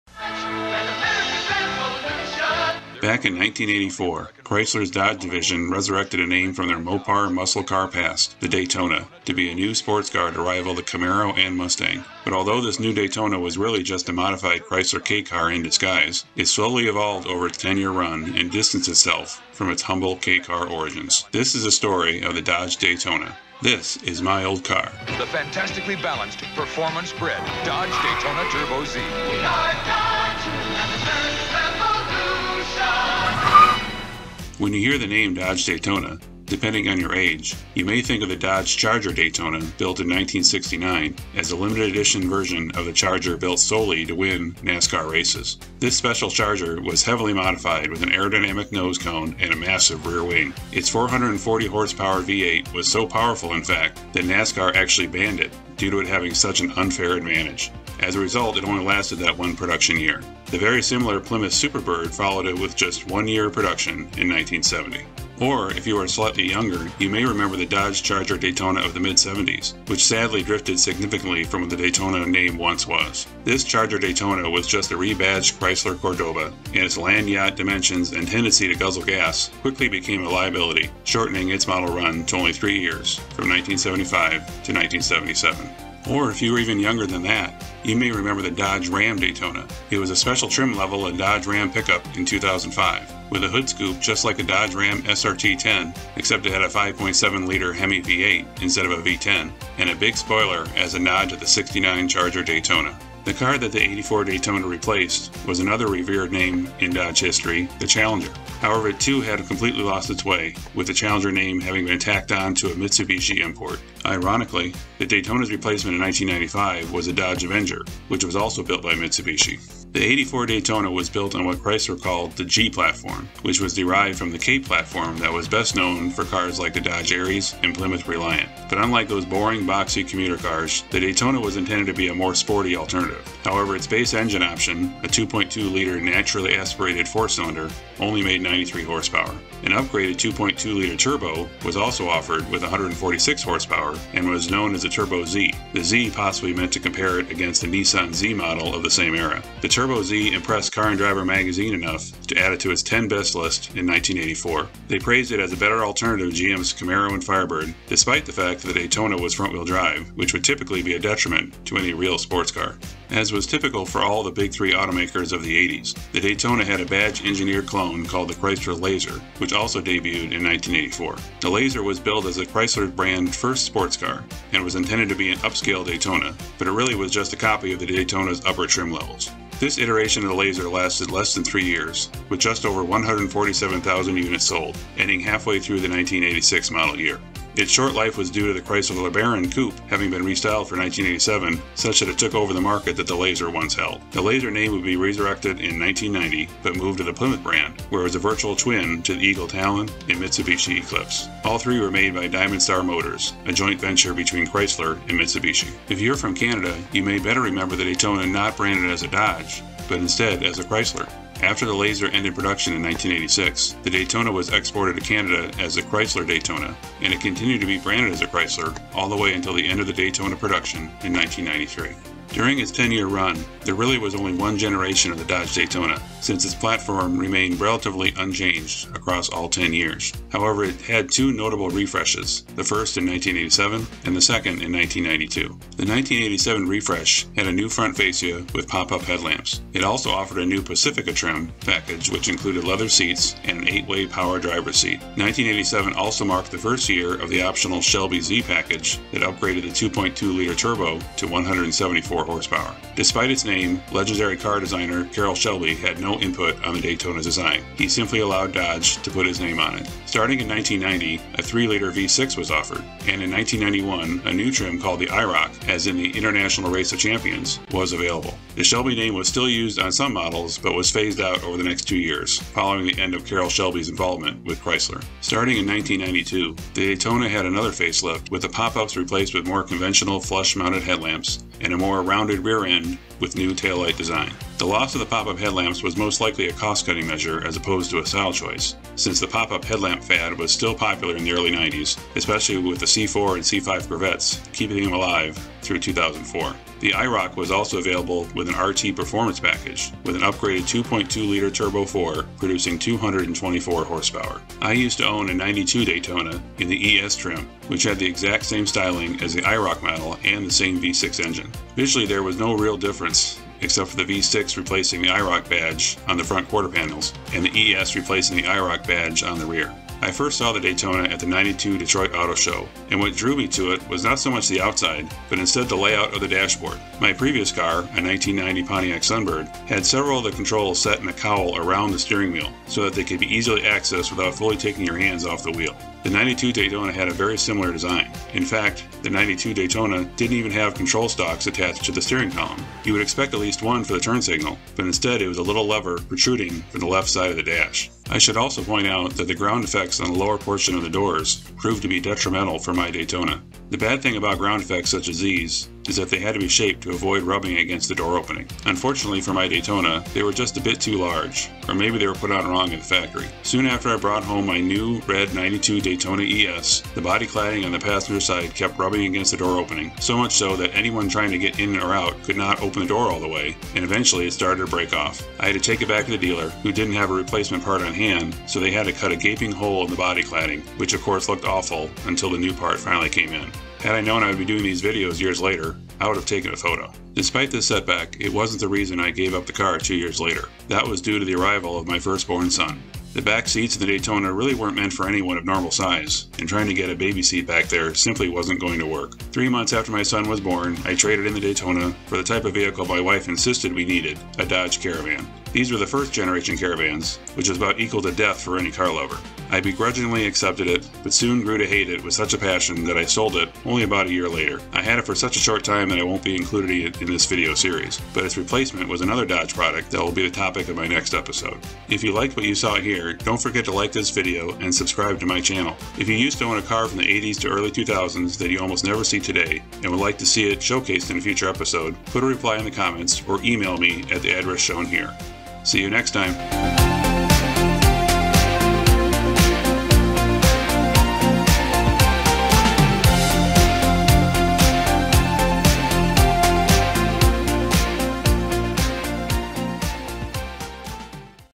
Here’s how the Dodge Daytona was an evolution of the Chrysler K-car
In this episode I provide the history of the Dodge Daytona that ran from 1984 to 1993, how it originated from the K-car, and list all of the other cars Dodge released before and after that also used the word Daytona in their name.

If you used to own a car from the 80's to early 2000's that you rarely see today, leave me a note in the comments and I may include it in a future video.